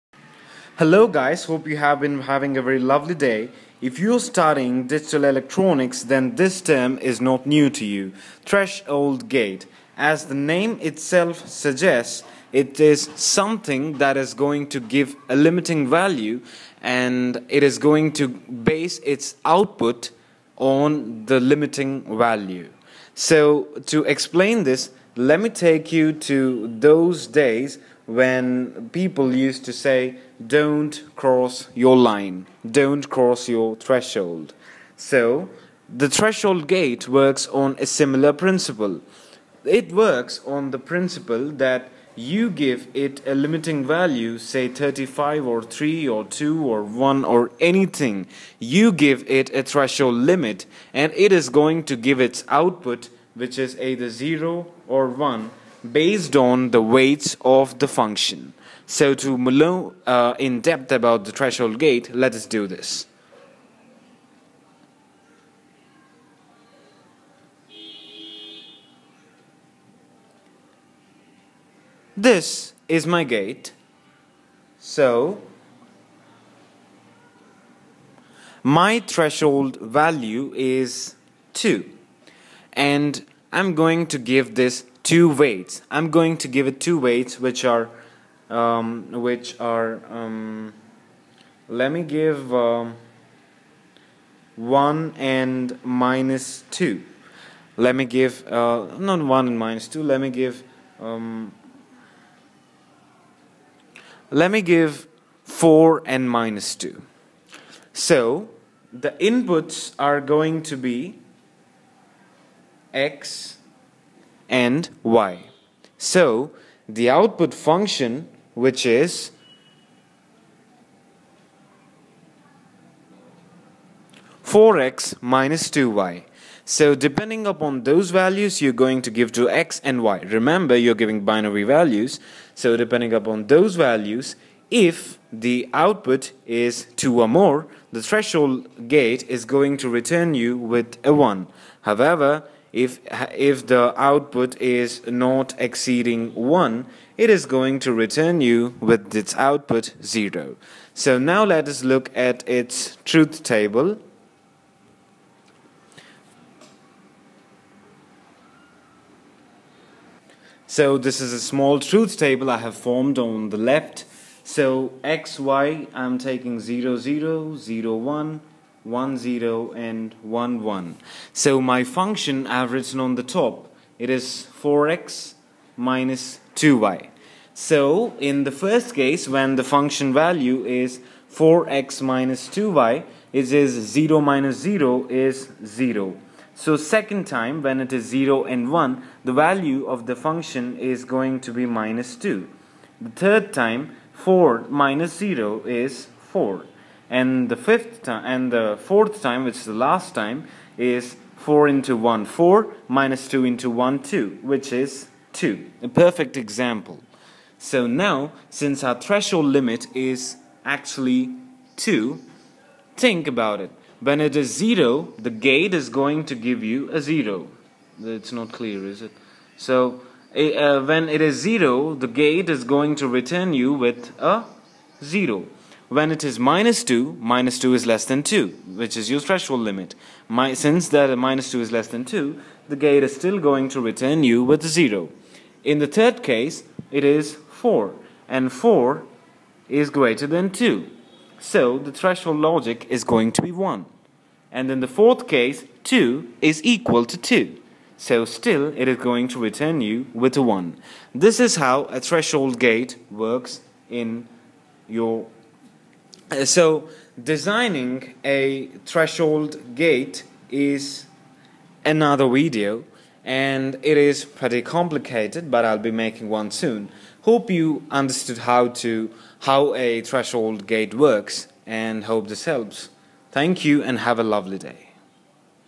Threshold Gate: Digital Electronics
This video clearly explains you how a threshold gate works along with a truth table.